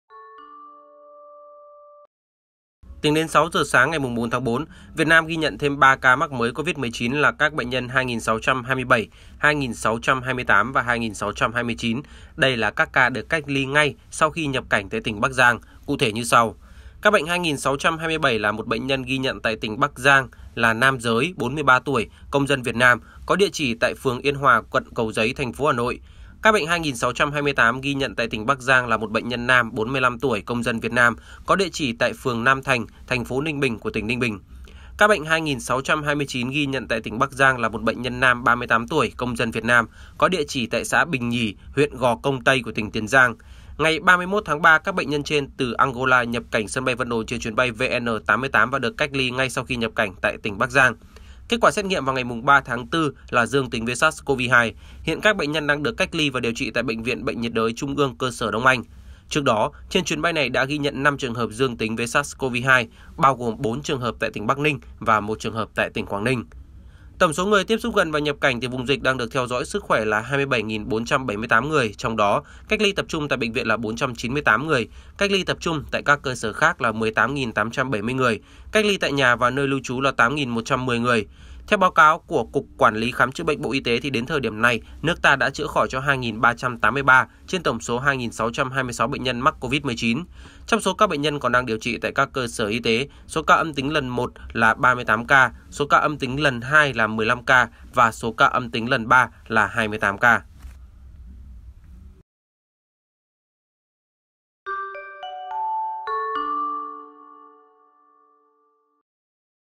Dịch Covid-19 hôm nay sáng 4/4: Bắc Giang ghi nhận 3 ca mắc Covid-19 mới | VTC1
VTC1 | Bản tin sáng 4/4 cuả Bộ Y tế cho biết có 3 ca mắc Covid-19 tại Bắc Giang. Đây là những trường hợp nhập cảnh đã cách ly ngay.